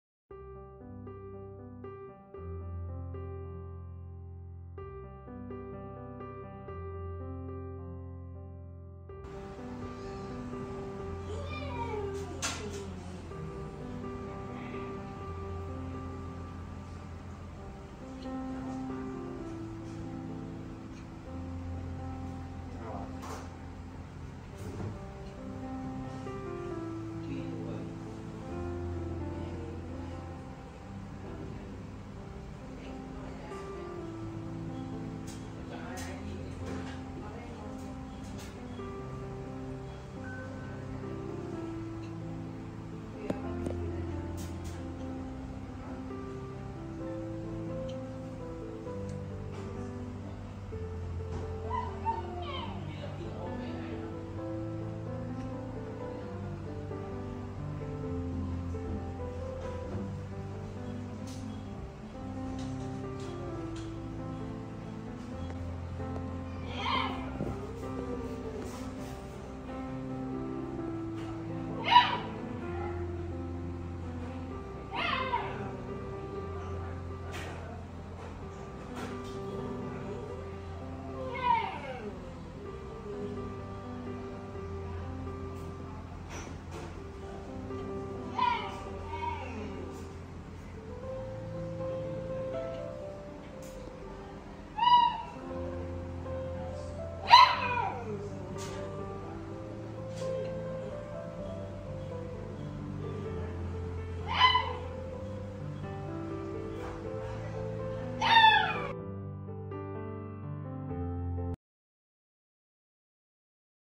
Update: The result of blood tests Rocco is not positive with FIP
Playlist link